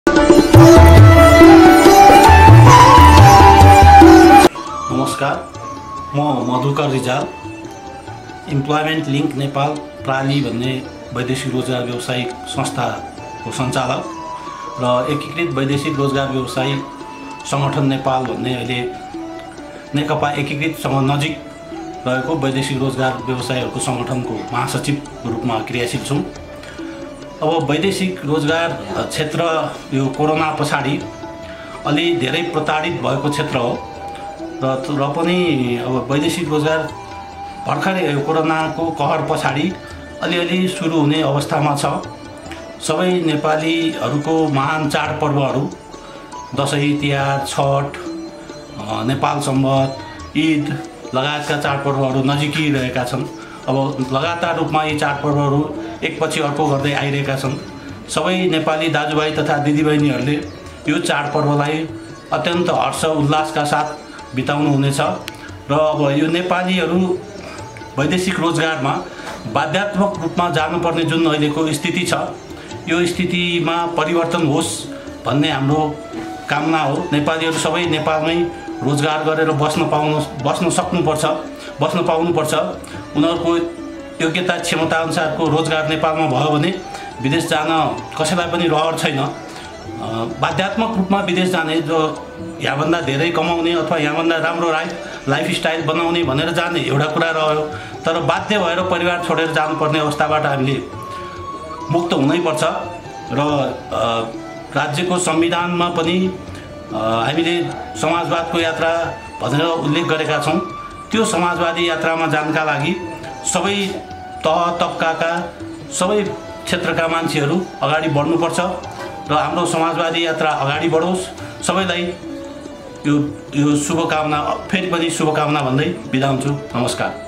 विजया दशमी तथा शुभ दिपावलीको शुभकामना|मधुकर रिजाल एकीकृत वैदेशिक रोजगार व्यावसायि सगंठन महासचिव
यश भिडियोमा विजया दशमी तथा शुभ दिपावलीको शुभकामना|मधुकर रिजाल एकीकृत वैदेशिक रोजगार व्यावसायि सगंठन नेपाल महासचिव